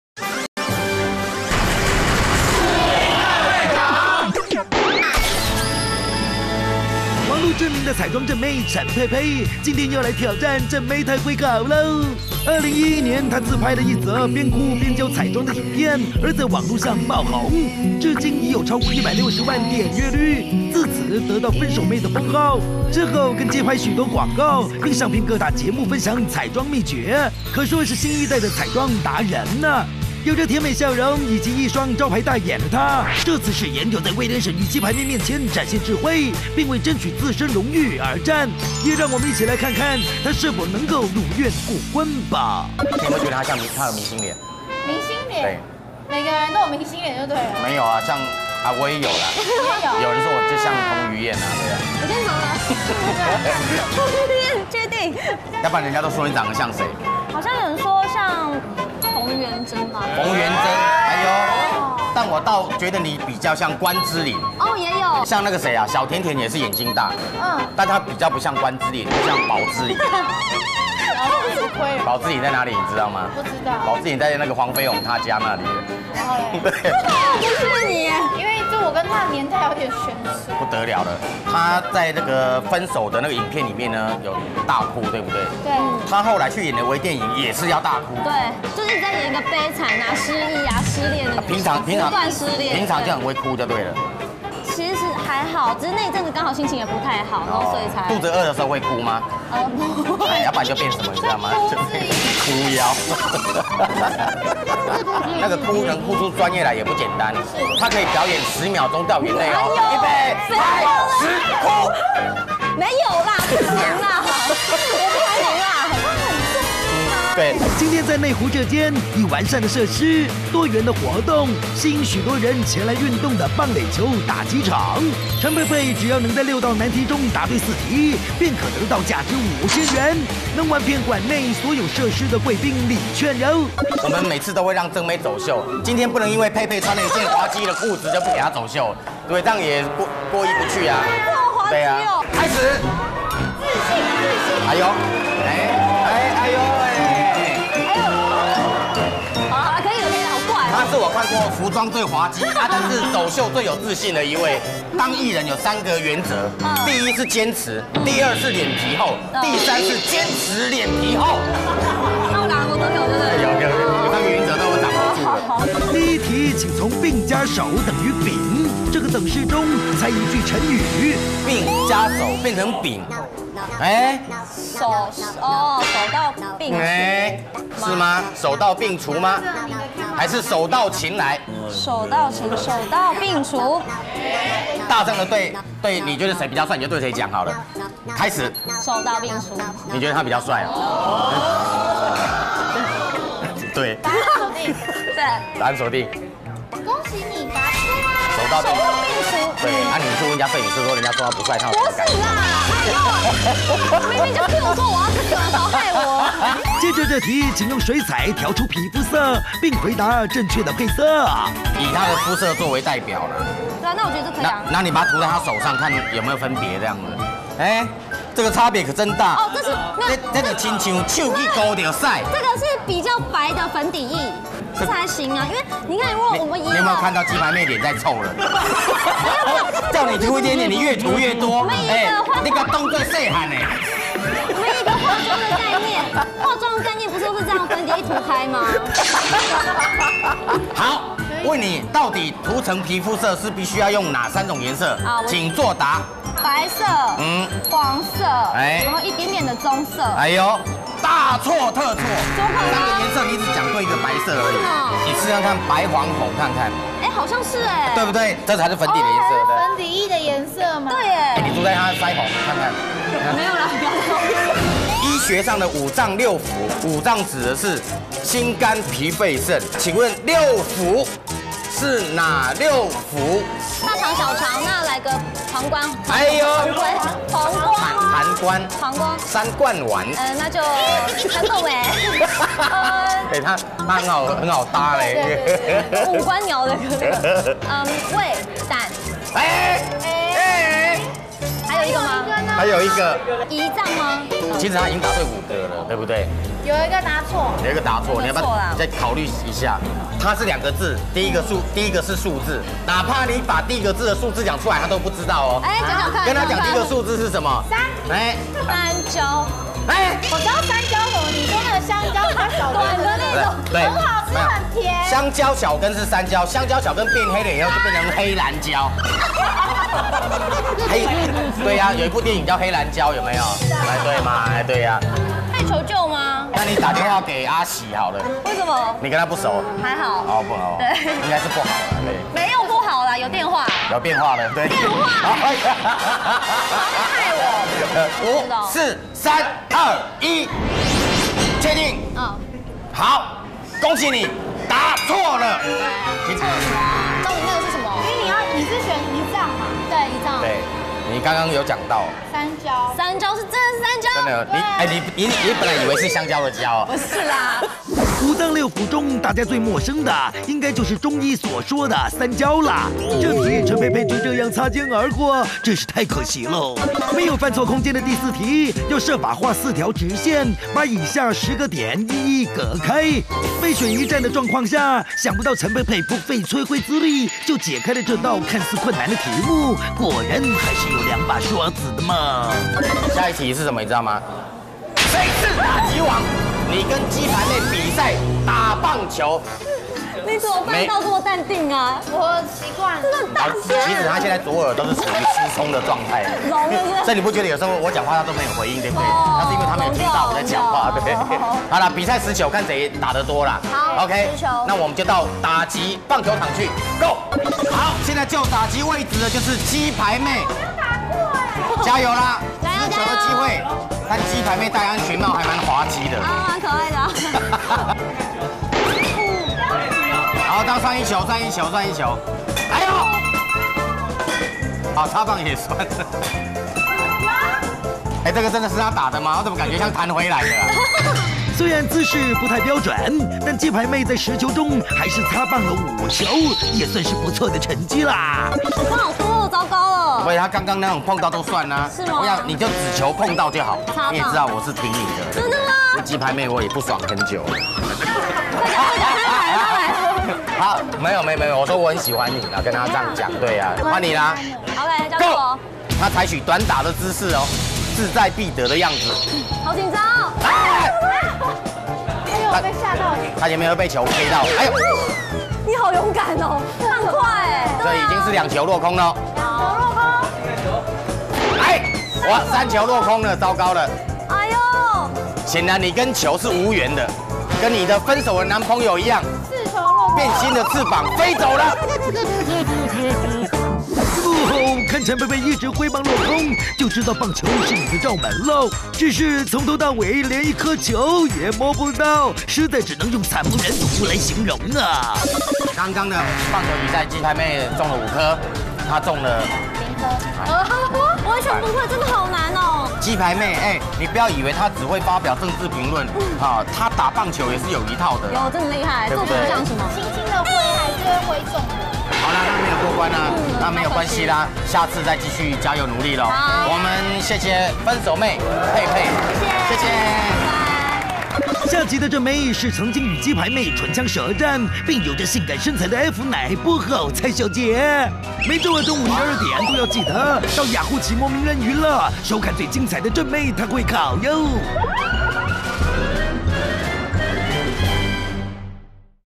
分手妝正妹 陳佩佩 把她當細漢 篇
分手妝正妹 陳佩佩 把她當細漢 篇 因「分手妝」爆紅的分手妹陳佩佩，不僅在網路擁有高人氣，更因此受邀演出微電影！這回遇到同樣在網路有高人氣的沈玉琳與雞排妹，除了大膽挑戰，竟然膽大包天的將雞排妹當做「細漢」，這...